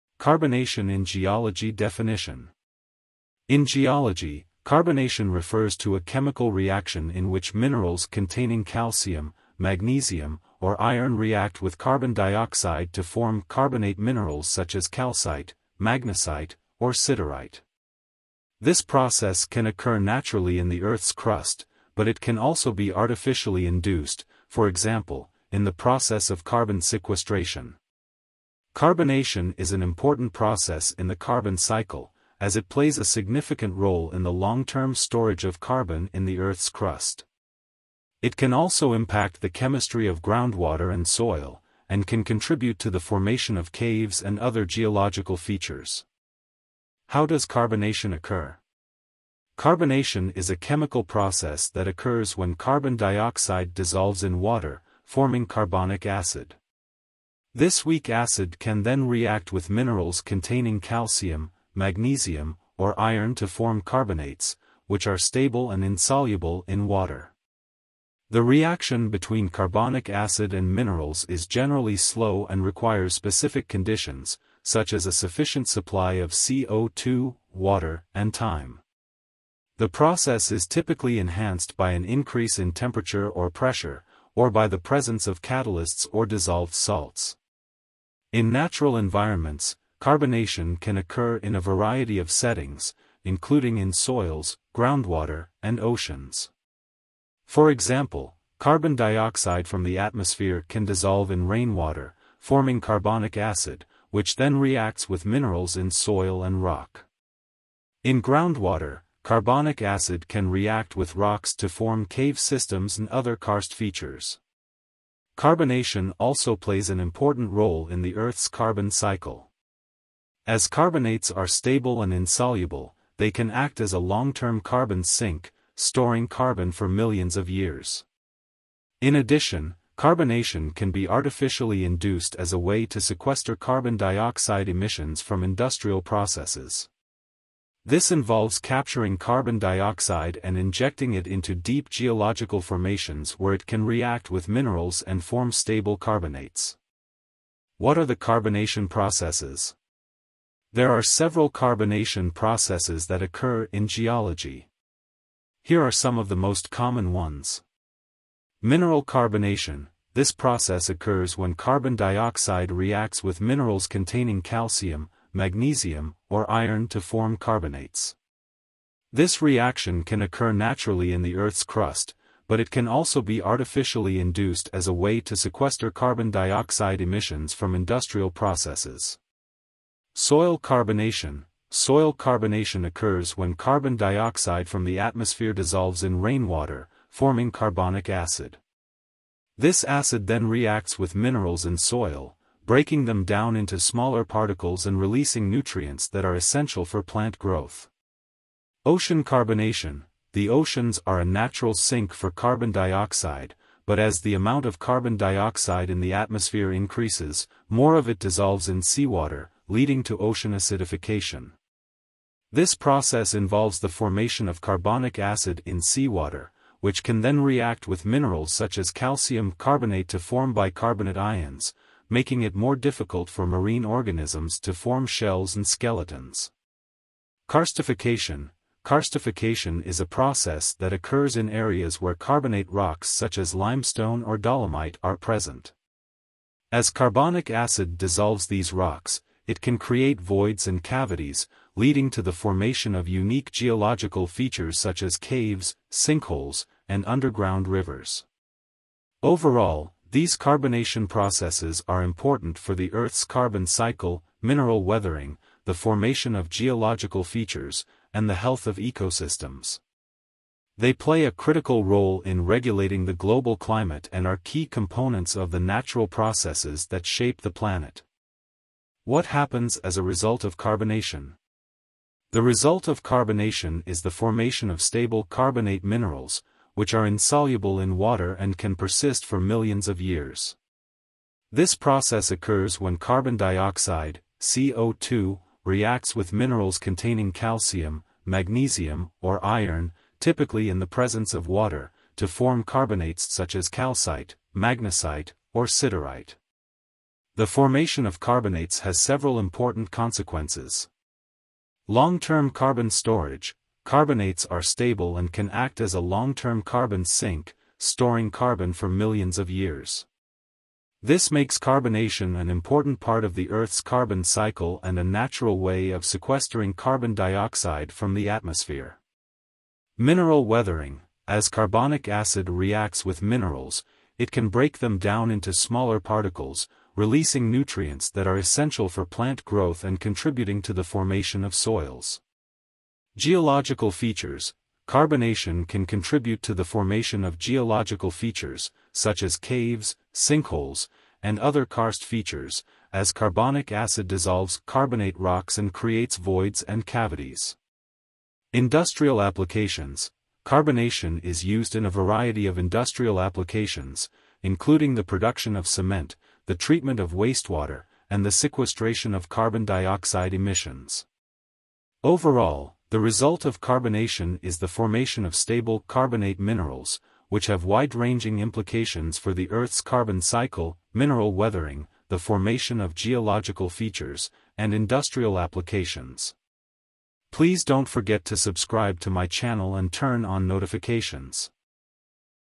What is carbonation? How does carbonation occur? What are the carbonation processes?
In this video, What is carbonation? , how does carbonation occur? What are the carbonation processes? What happens as a result of carbonation? detailed information is given.